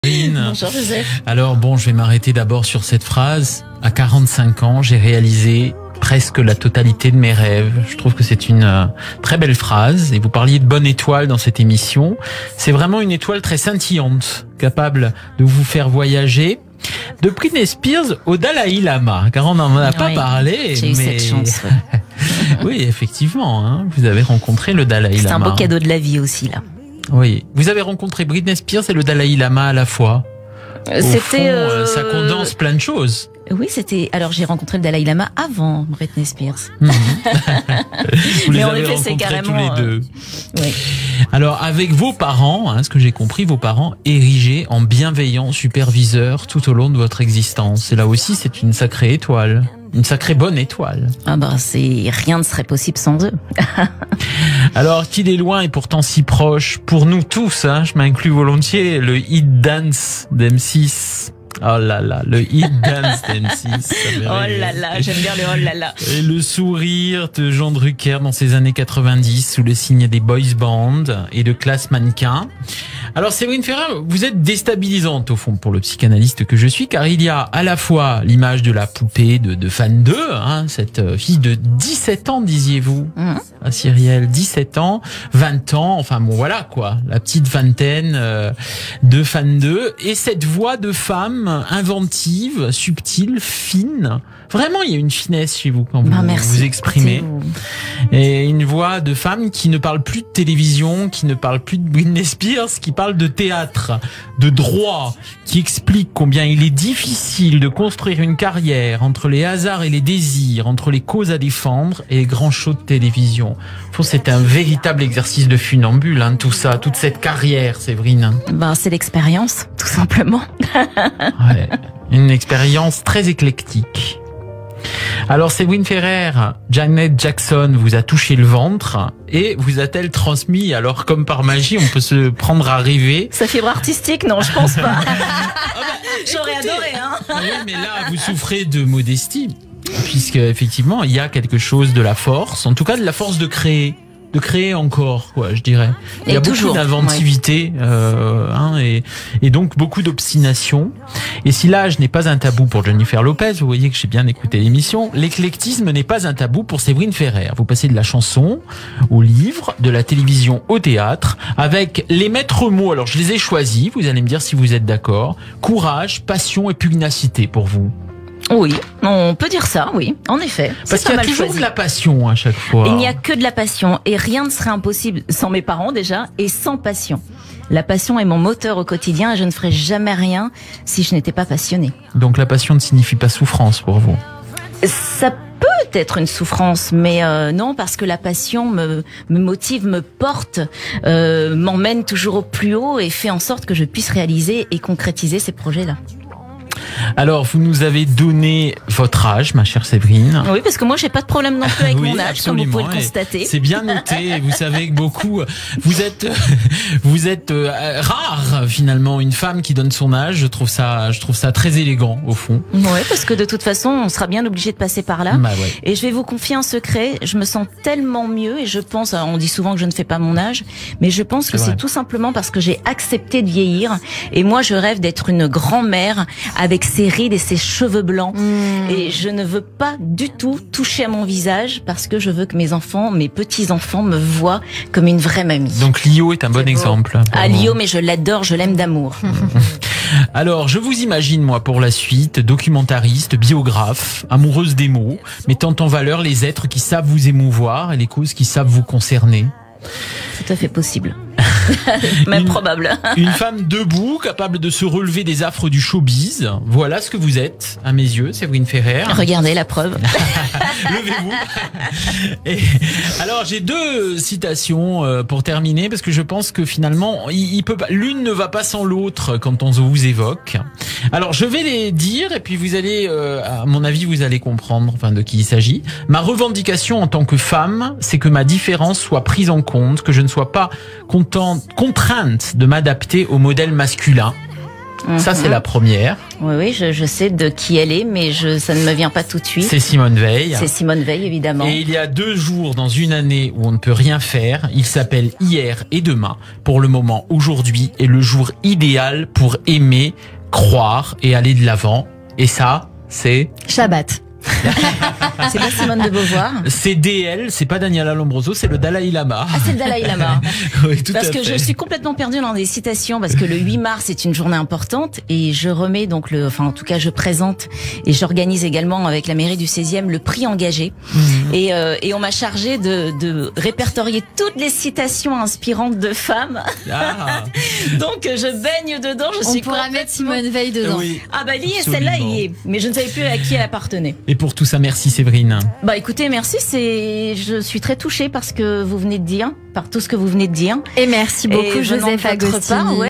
Le portrait psychologique de Séverine Ferrer par Joseph AGOSTINI
Joseph Agostini dresse le portrait psy de l’animatrice Séverine Ferrer